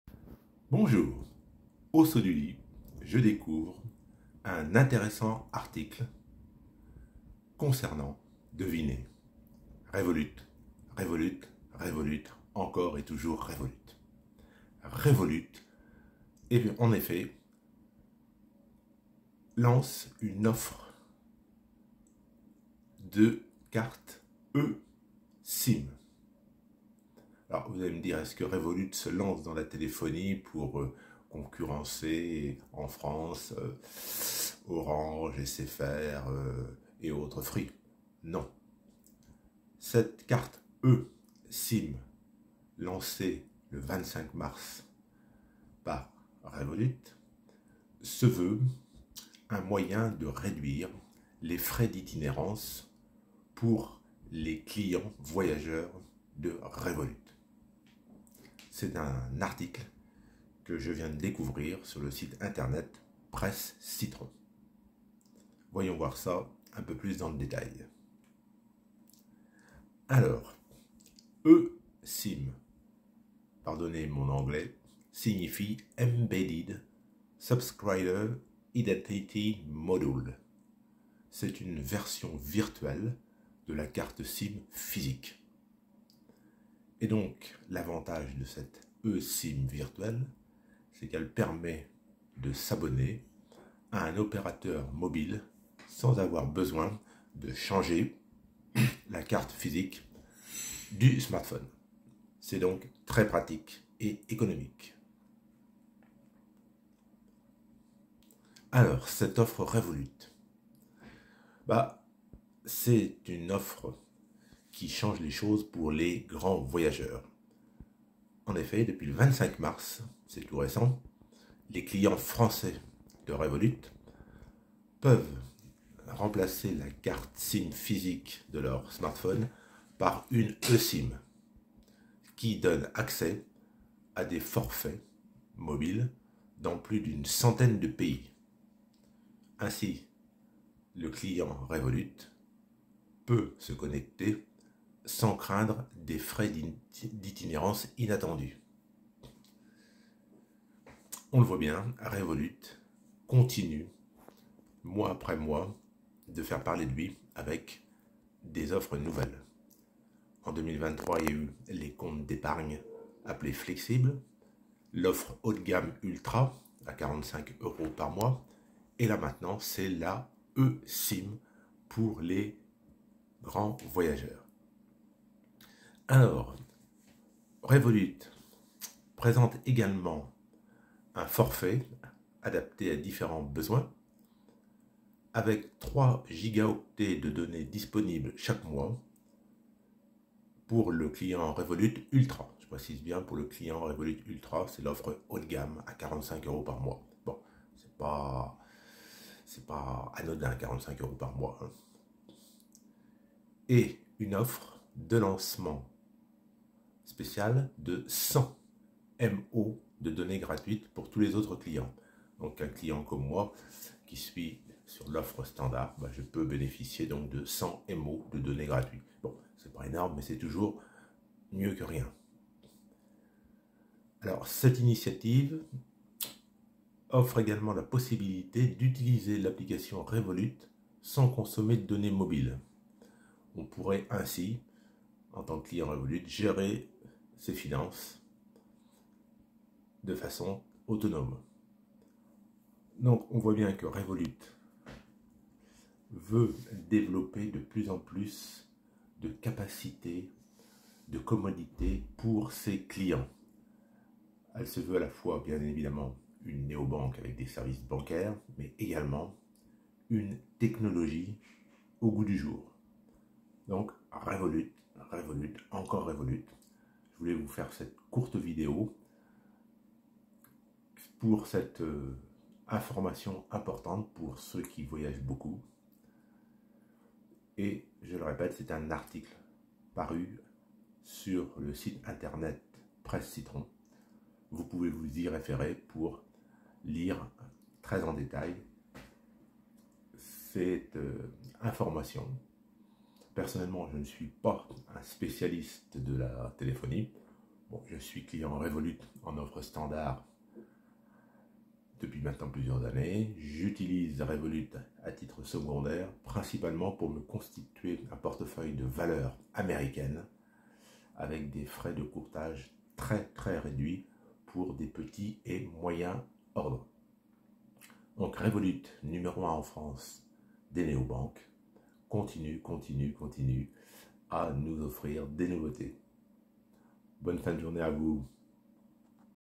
Revolut, carte eSim pour réduire les frais d'itinerance des grands voyageurs
Revolut continue d'offrir des services innovants.